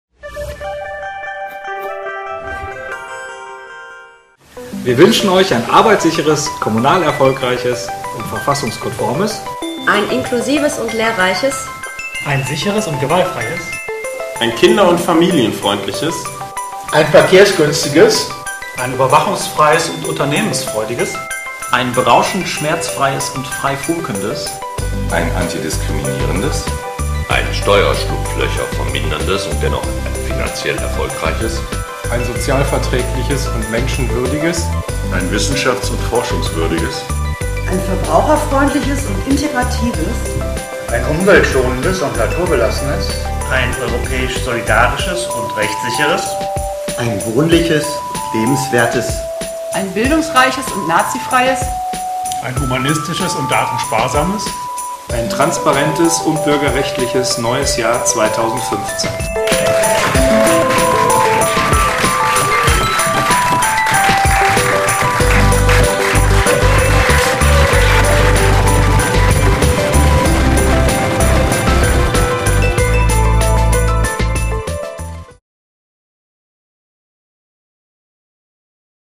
"Alles Gute für 2015" sagen die Piraten im Landtag NRW
Unsere ganz speziellen Neujahreswünsche für die Menschen in NRW!